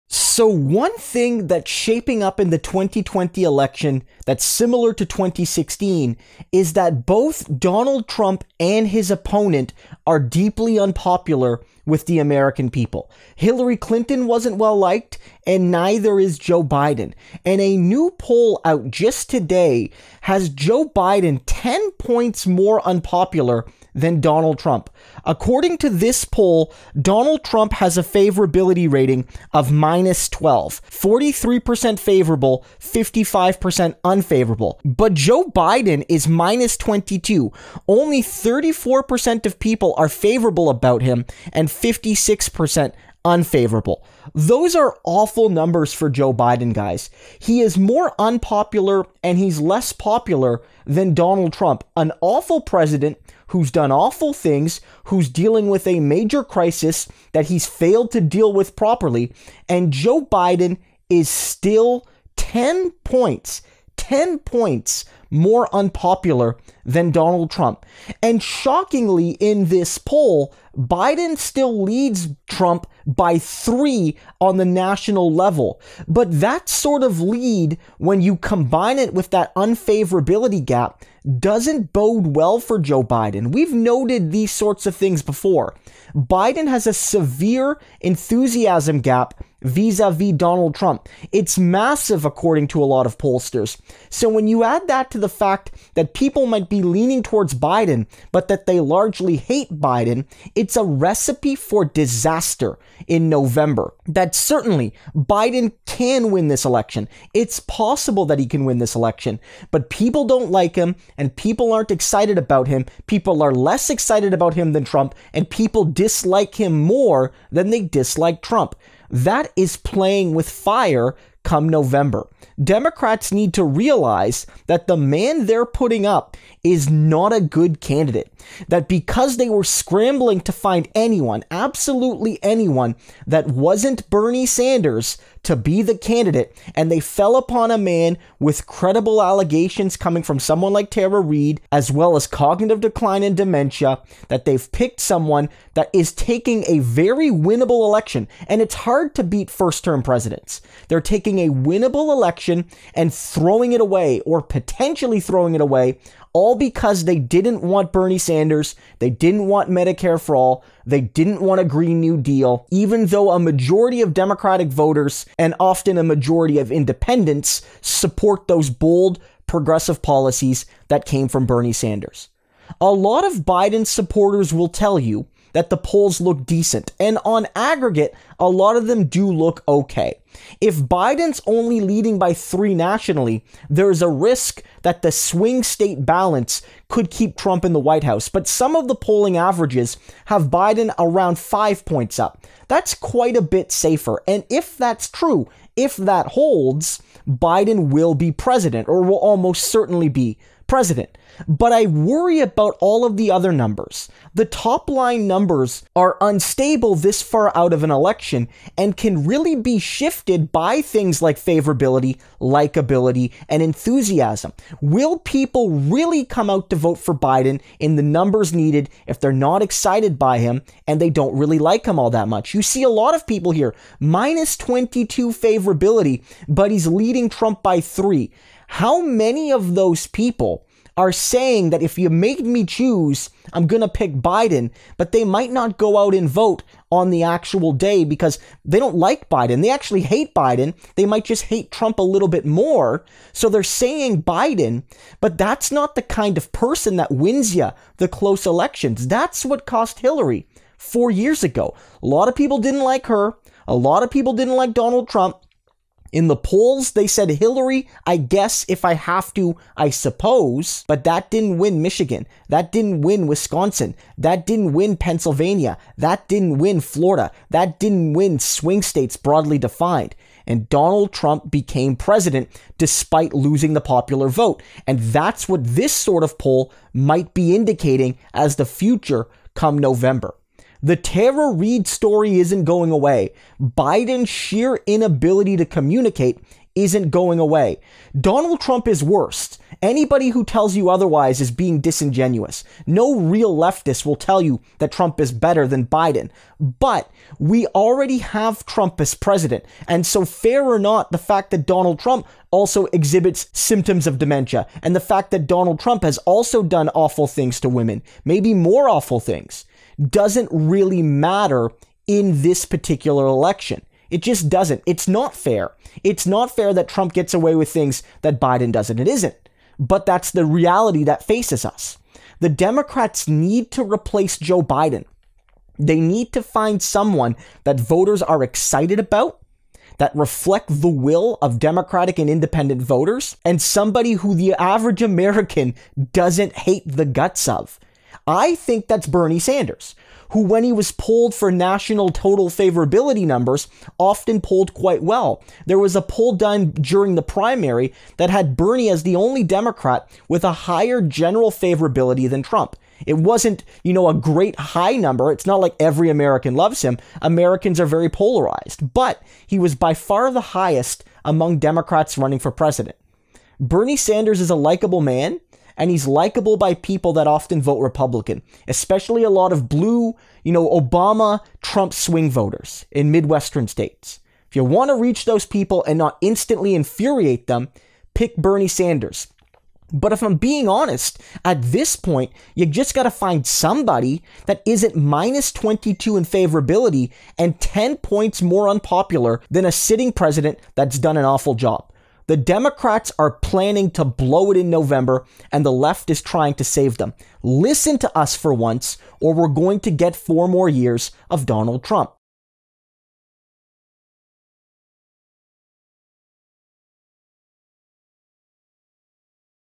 New poll shows Joe Biden more hated than Donald Trump
A new Civiqs poll shows that while Joe Biden is beating Donald Trump by 3 points nationally (47-44), it shows him with a MUCH lower favorability rating. Trump is -10 while Biden is -22. This, combined with Republican advantages in enthusiasm and in the electoral college means Biden is in real danger of losing the election.

Like in 2016 with Hillary Clinton, they picked an unpopular candidate who didn't excite voters And the result was a disastrous loss to Trump. Tara Reade isn't going away, neither is Biden's dementia and cognitive decline, nor his rejection of Medicare for All.

If Democrats want to win, they need to replace Biden with Bernie Sanders or anyone who isn't hated by the electorate.